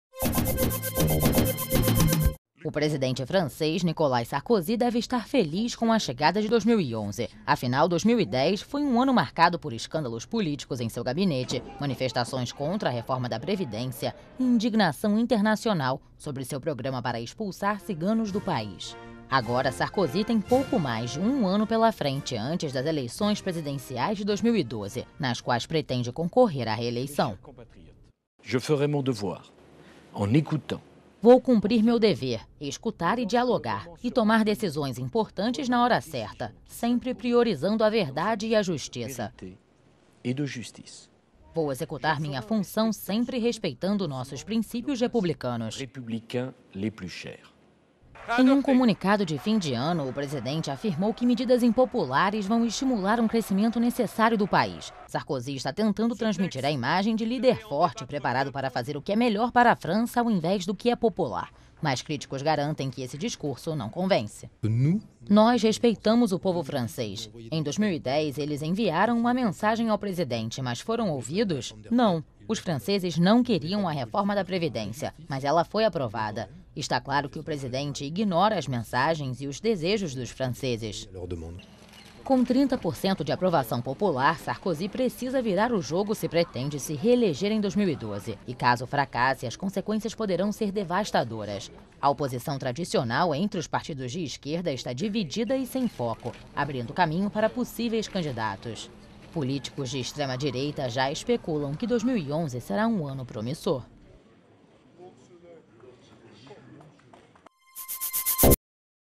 2011: ano decisivo para Sarkozy
Depois de um ano de escândalos políticos, protestos e baixa popularidade, o presidente francês Nicolas Sarkozy está feliz com a chegada de 2011. Em 2012 serão realizadas as eleições presidenciais e, até lá, de alguma forma, o líder espera reverter sua imagem política e reconquistar os franceses.